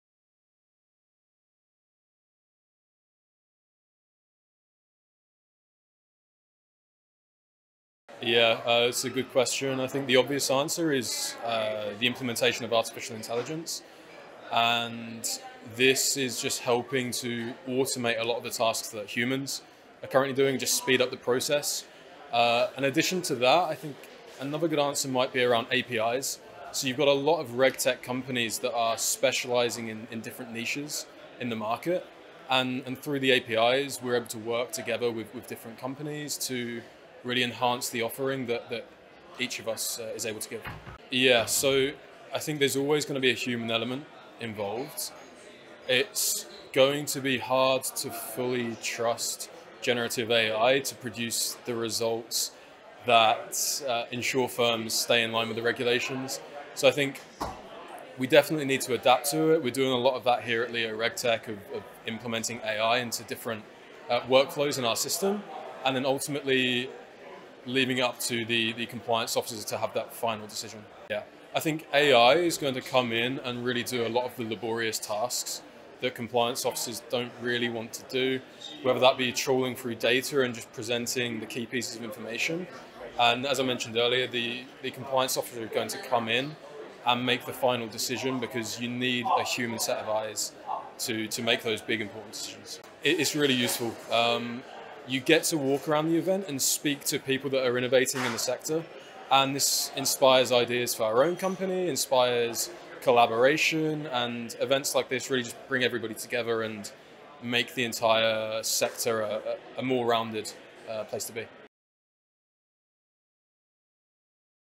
Global RegTech Summit 2024 - Luke Shepherd, Leo RegTech
Interview with Luke Shepherd, Director - Business Development at Leo RegTech at the Global RegTech Summit 2024 hosted by FinTech Global